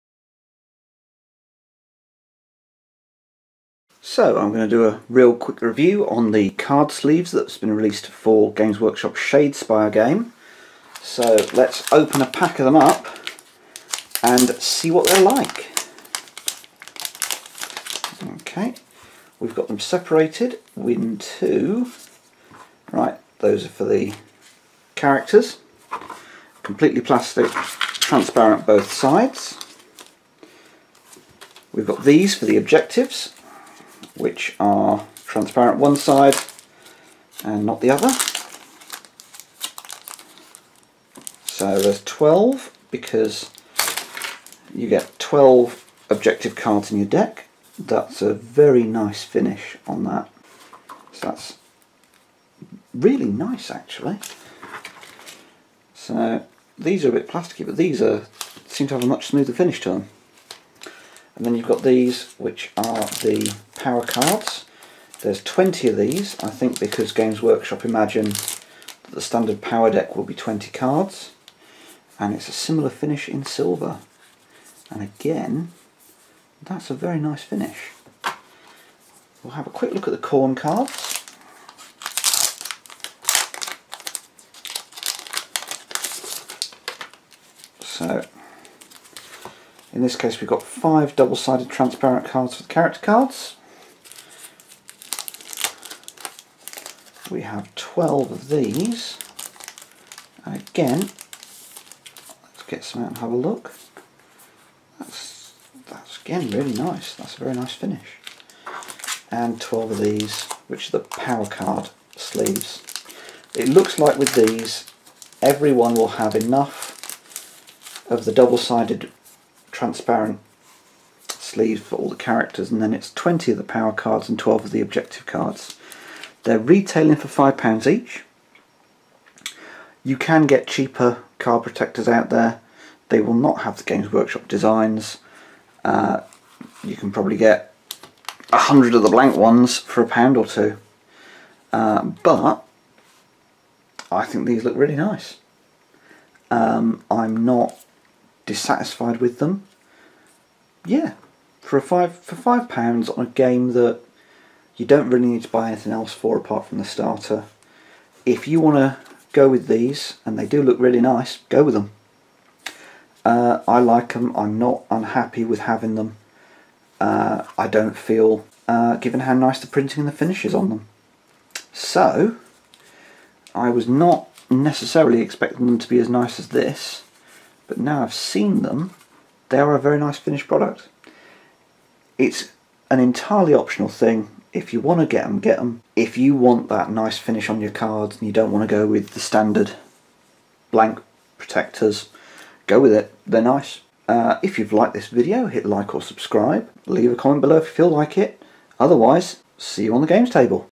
Shadespire Card Sleeves
A quick review of the Shadespire card sleeves released by Games Workshop.